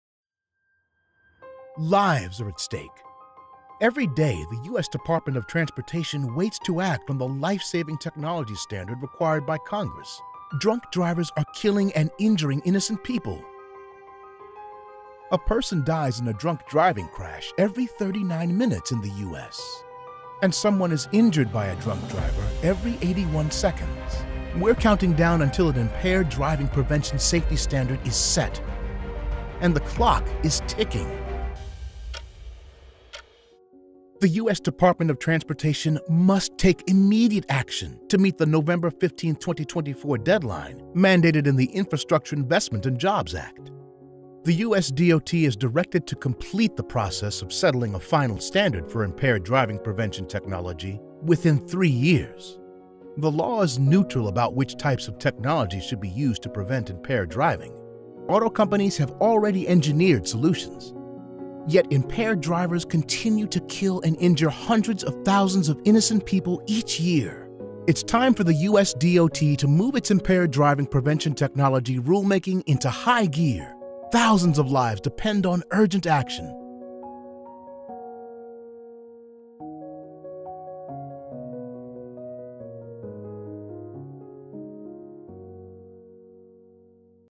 Auto technology to prevent impaired driving
It’s been two years since the Infrastructure Investment and Jobs Act was signed into law, with a provision that requires a national safety standard for state-of-the-art smart technology in all new cars that would ultimately eliminate impaired driving. The law requires USDOT to issue a final rule before November 2024 and gives auto makers two to three years after that to equip new cars with technology that complies with the USDOT rule.